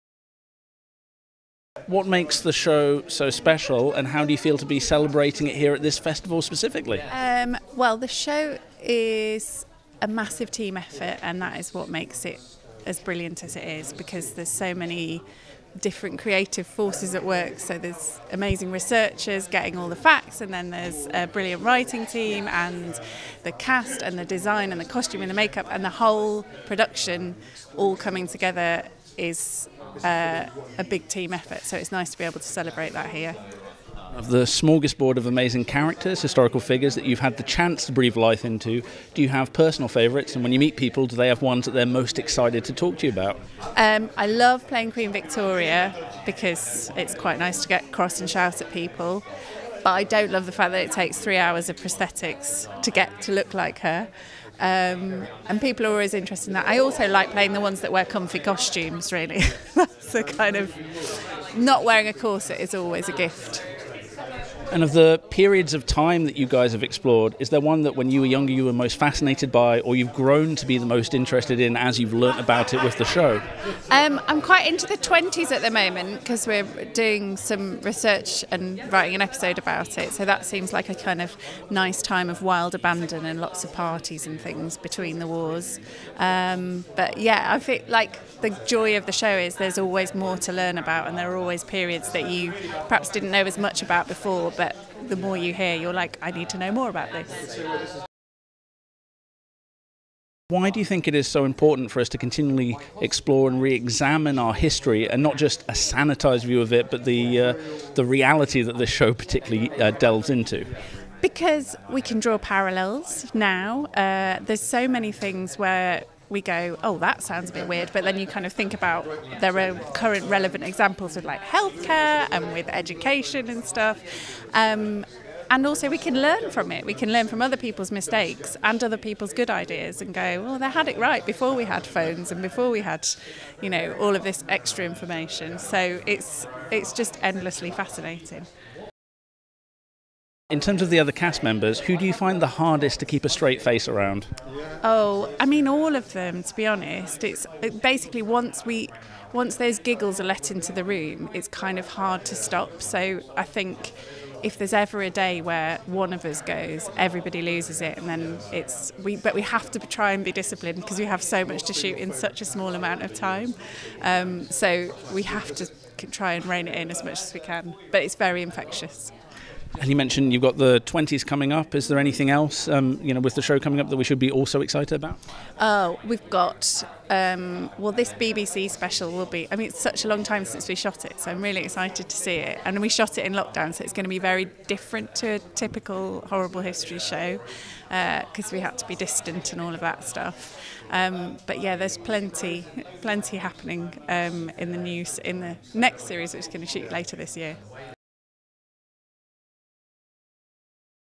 Jessica Ransom Interview Horrible Histories
Red Carpet News TV talks to VIP guests at the BFI Radio Times TV Festival 2022.

Featured shows include Call The Midwife, Toast of London, All Creatures Great and Small, Starstruck, It’s A Sin, His Dark Materials, Conversations With Friends, Horrible Histories, Heartstopper and The Outlaws.